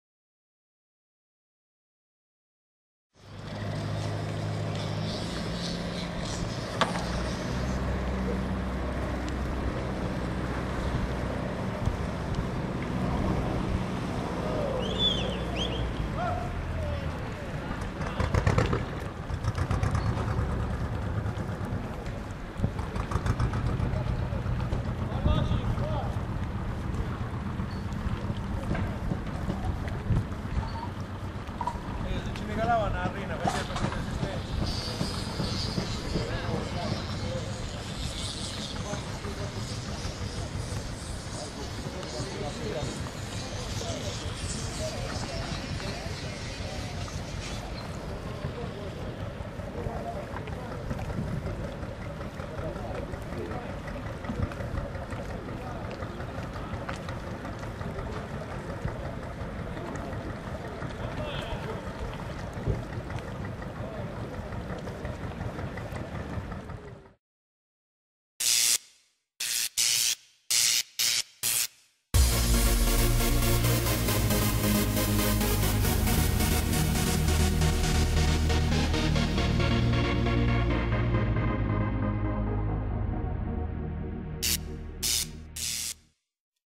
Varo alla Balata
Molti di noi sono al corrente di come si "getta" in acqua una barca, ma avete mai visto quale procedura si attua nella nostra Marzamemi? Eccone un esempio...